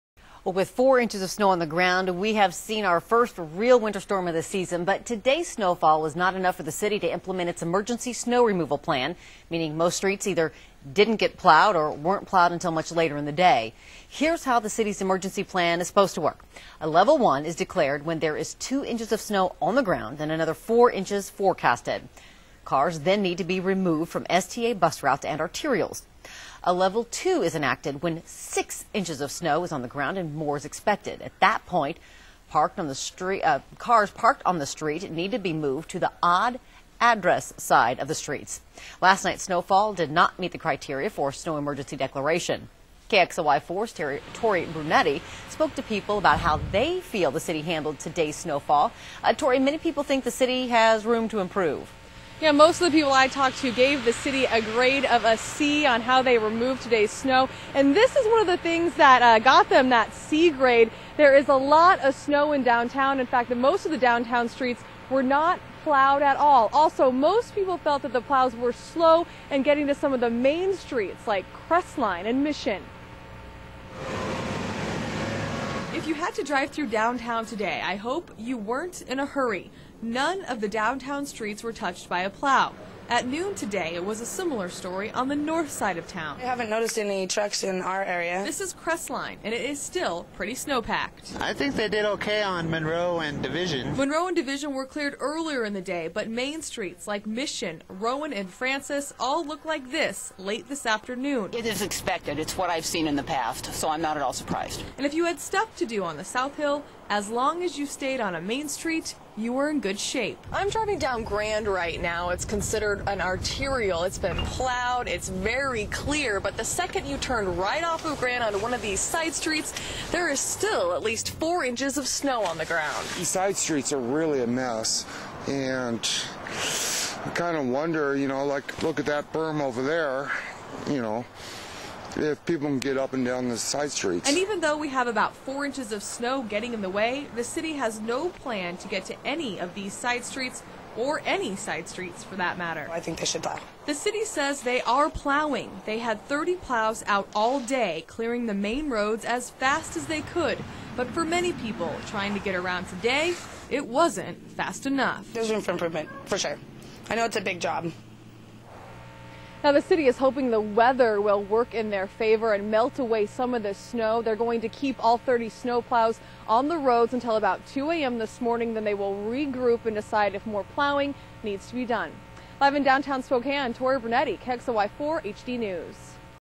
City gets a C from its citizens
Last nights snowfall didnt meat the criteria for a snow emergency declaration and the people of Spokane werent shy to say how they felt about how the city handled Tuesdays snow. KXLY4's Tori Brunetti reports.